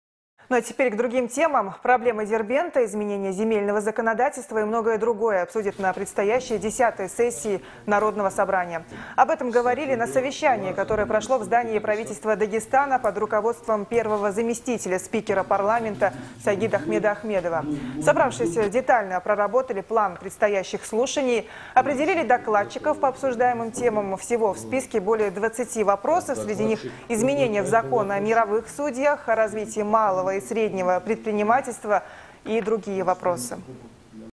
Первый вице-спикер НС Сайгидахмед Ахмедов провел заседание Президиума
Проблемы Дербента, изменения земельного законодательства и многое другое обсудят на предстоящей 10 сессии Народного Собрания. Об этом говорили на совещании, которое прошло в здании правительства Дагестана, под руководством первого заместителя спикера парламента Сайгидахмеда Ахмедова.